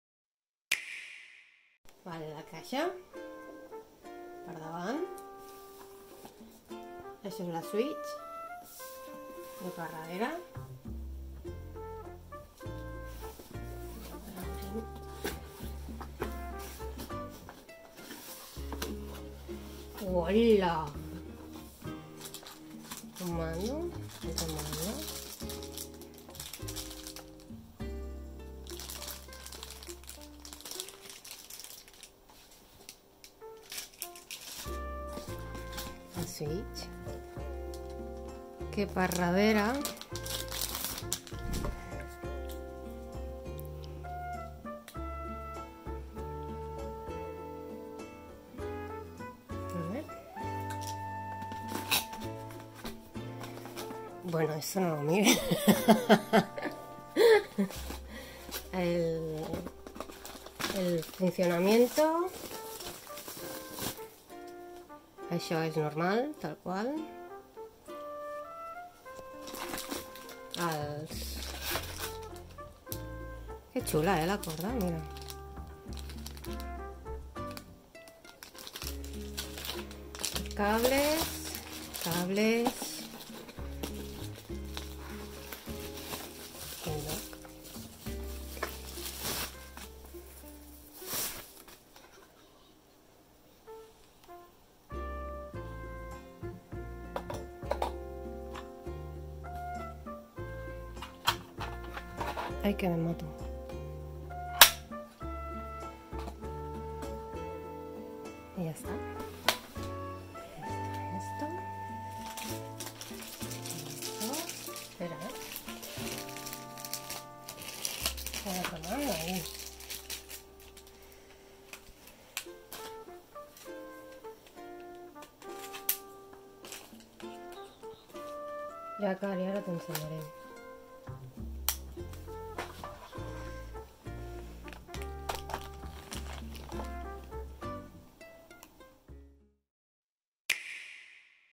[NTH UNBOXING] Nintendo Switch edició Animal Crossing: New Horizons
Amb motiu del llançament d'Animal Crossing: New Horizons per a Nintendo Switch, la companyia nipona va posar a la venta una consola híbrida d'edició especial inspirada en els motius d'aquesta infinita aventura. En aquest nou vídeo per al canal de Youtube de NintenhypeCat, la Marta us ofereix un unboxing de dita consola per a que observeu en detall tots els elements que incorpora!